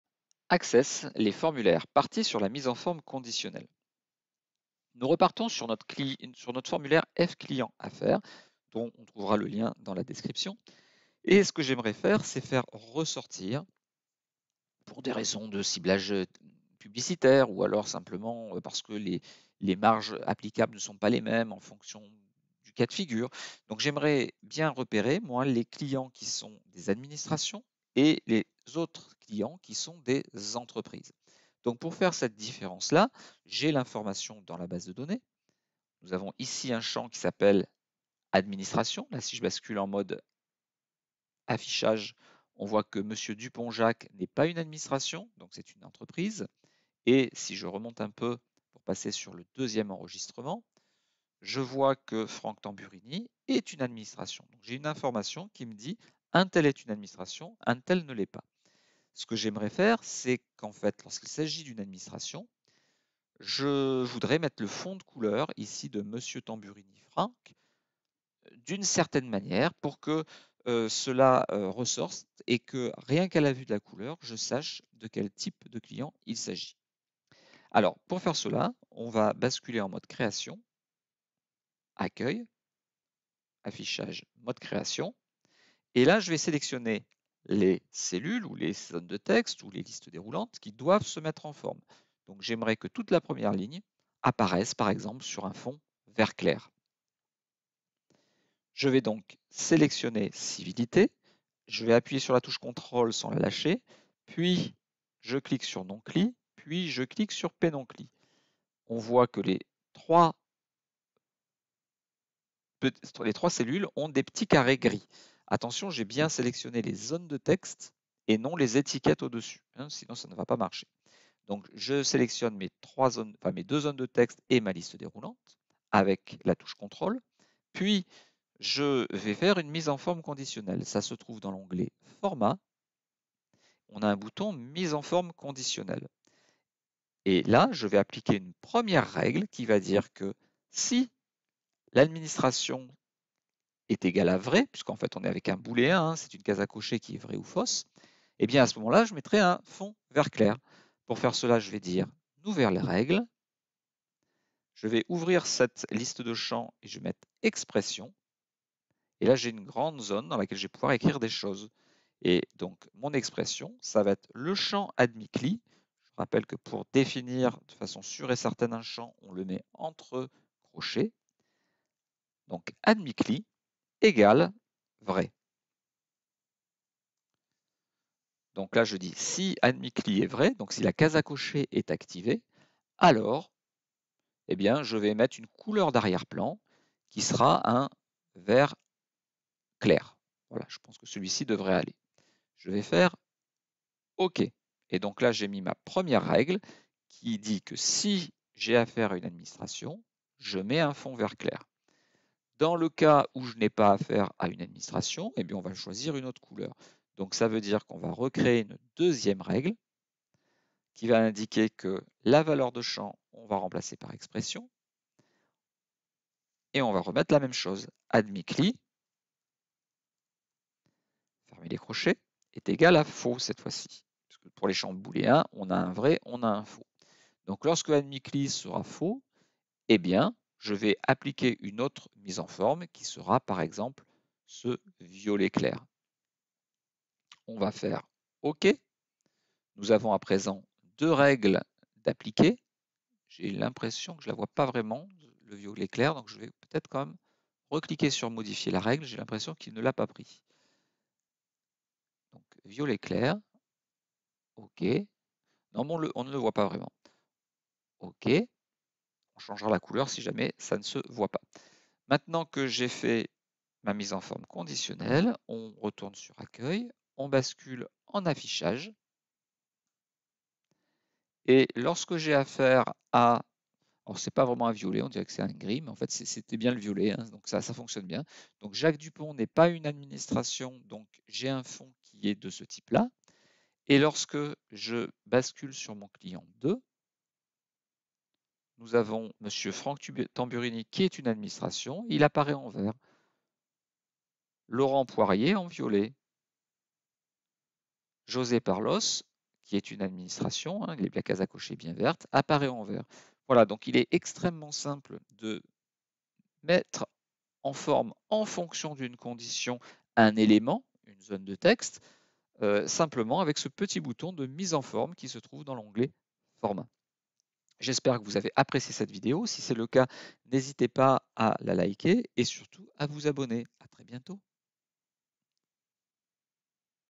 La mise en forme conditionnelle (Formulaires Access 21)
Faire ressortir certains éléments d'un formulaire en fonction d'une condition